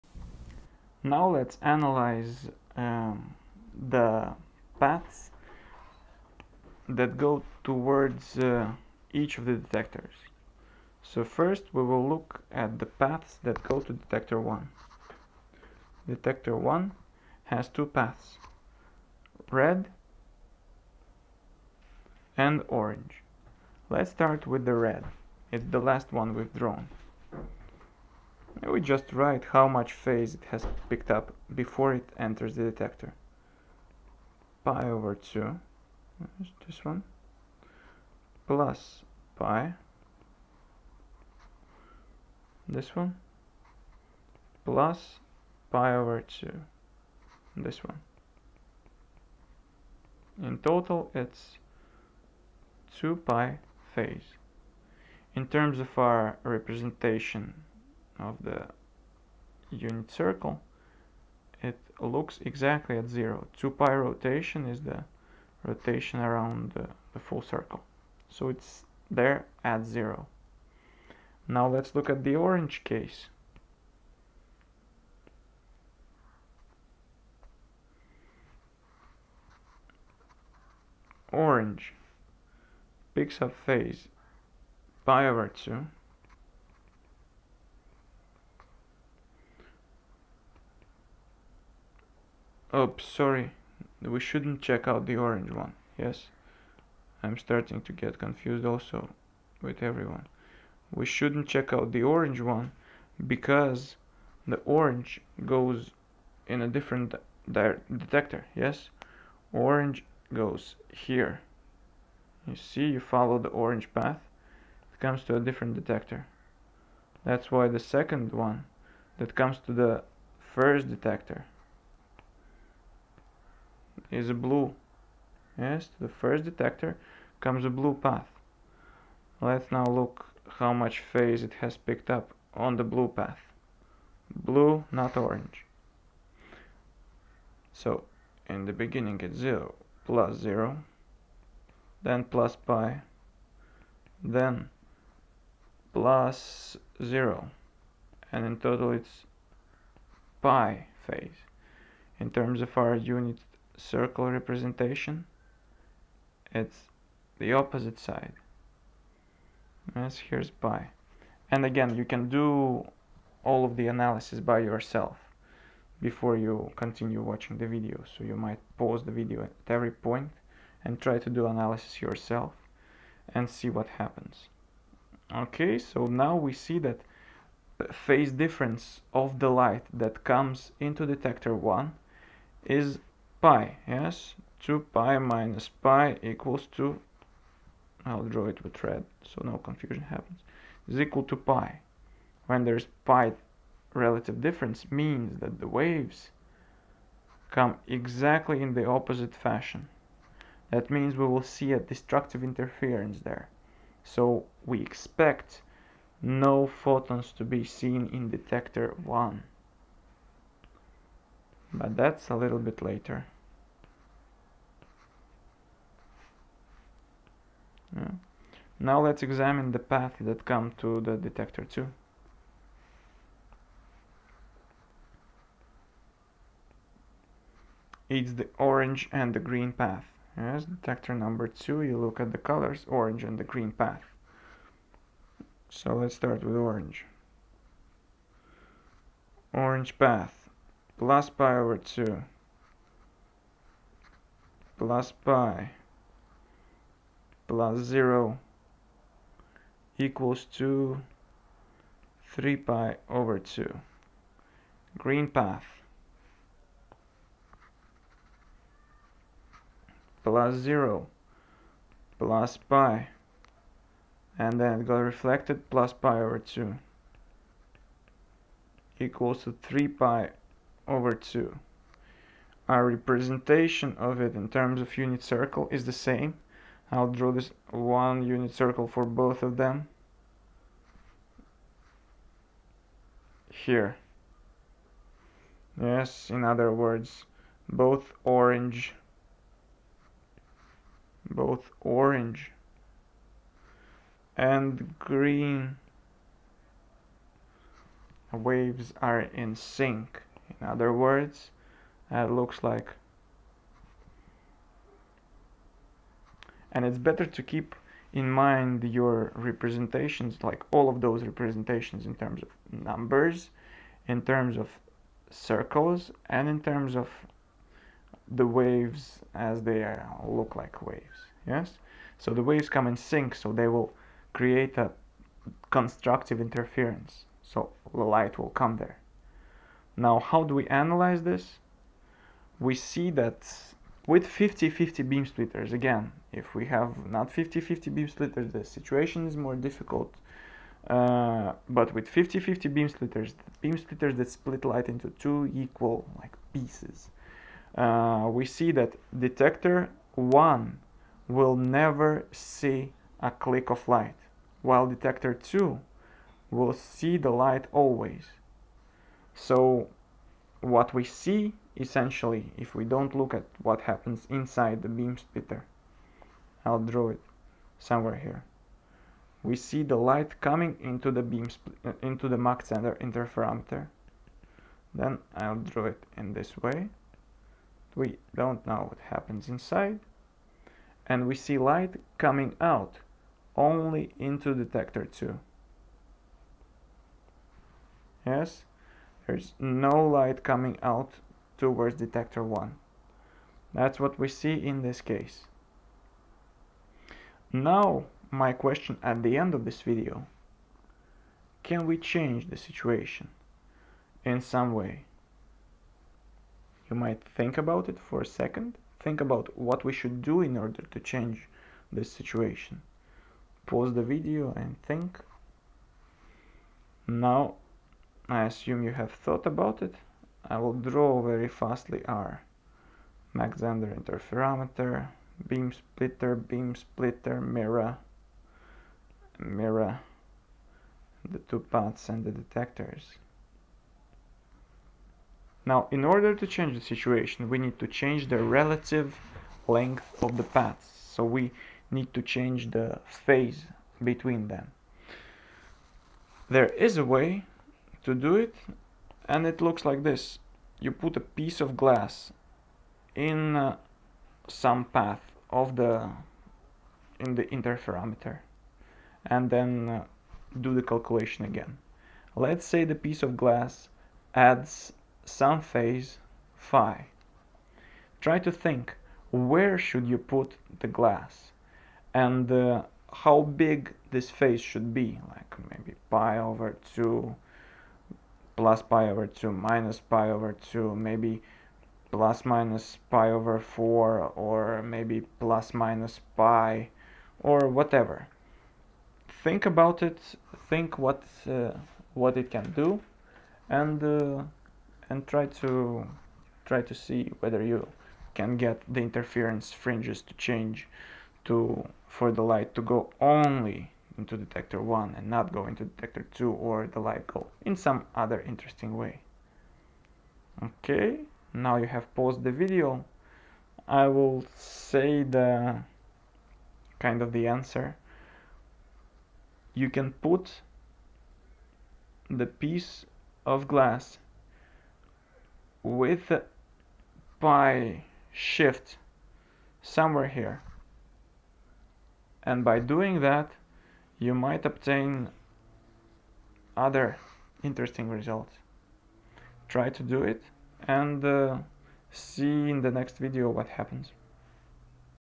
MZ interferometer. Analysis continued.
We finalize our analysis and see the interference.